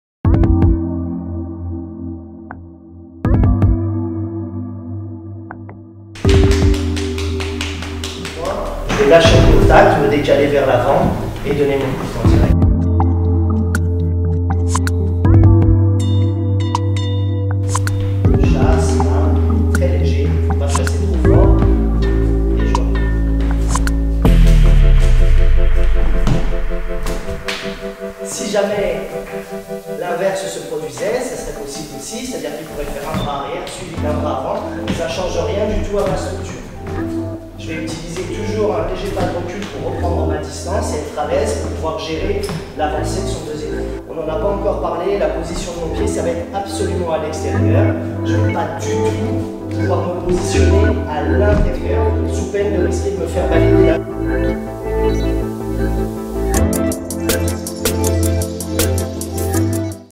Les TUTOS du WING CHUN - PAK SAO - Trailer 1 - HD 720p
Voici les premiers extraits de nos tutoriels d'apprentissage. Vous découvrirez dans le premier épisode une technique fondamentale pour l'interception du coup de poing direct, PAK SAO, la poussée de la paume.

N'oubliez pas de commentez et de likez la vidéo pour nous aidez à faire connaître notre travail. Quelques mots suffisent. Merci à vous